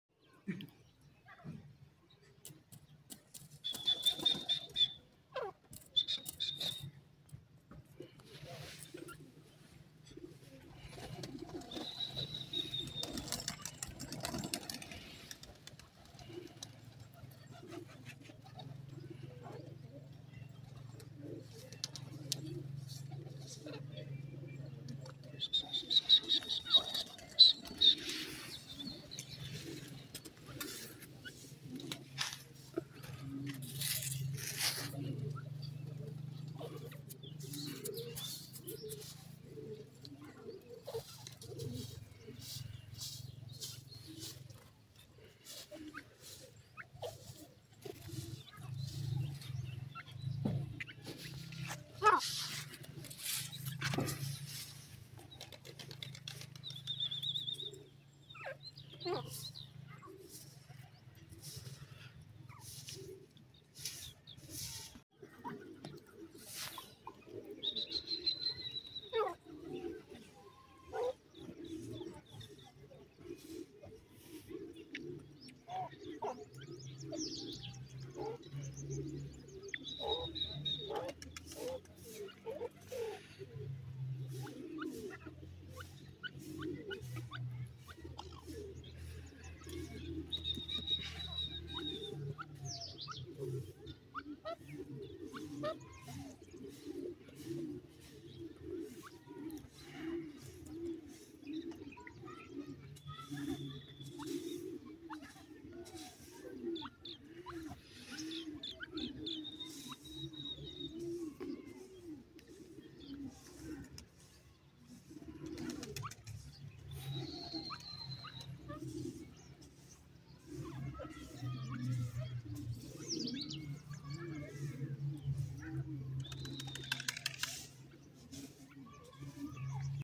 gora teetar jore breeder tiyar ha andon pr,teetar awaaz
In this video, we dig into the complexities of reproducing Gora Teetar matches, from choosing the ideal rearing accomplices to really focusing on the eggs until they hatch
 Furthermore, we've included valid accounts of Gora Teetar requires a genuinely vivid encounter